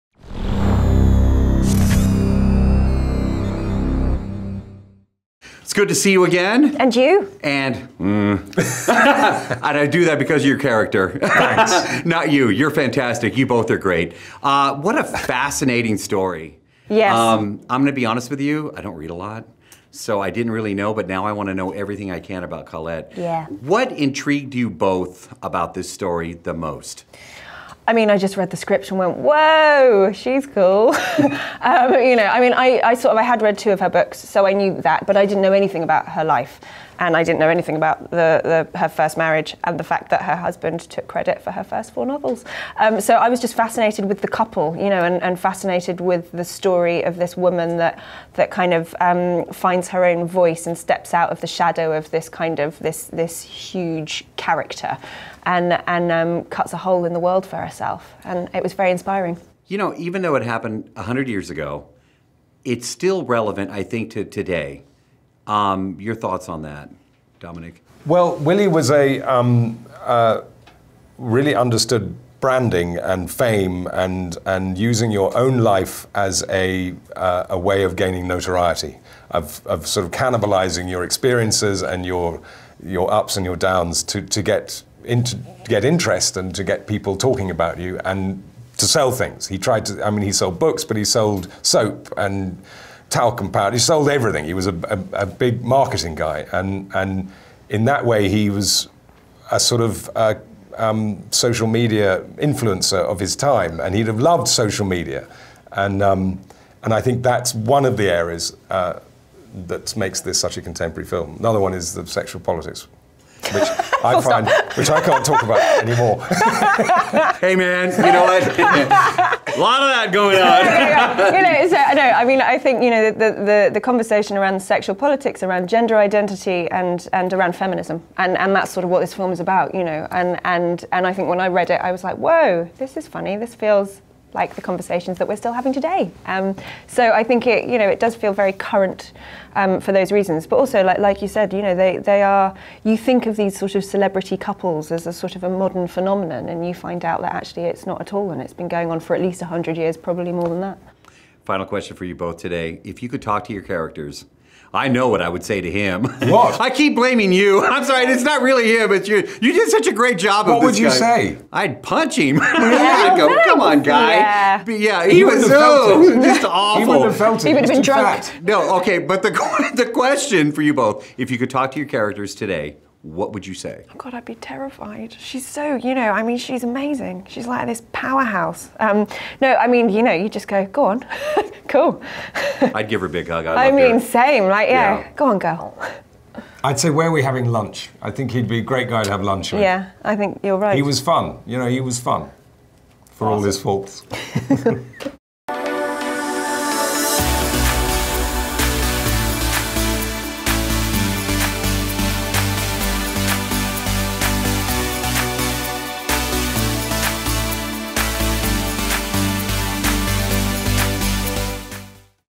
Keira Knightley & Dominic West on COLETTE & Sexual Politics
Keira Knightley and Dominic West sit down with Dave Morales to talk about COLETTE and the real-life sexual politics that were involved in the film.

Synopsis:

After moving to Paris, author Sidonie-Gabrielle Colette agrees to ghostwrite a semi-autobiographical novel for her husband. Its success soon inspires her to fight for creative ownership and overcome the societal constraints of the early 20th century.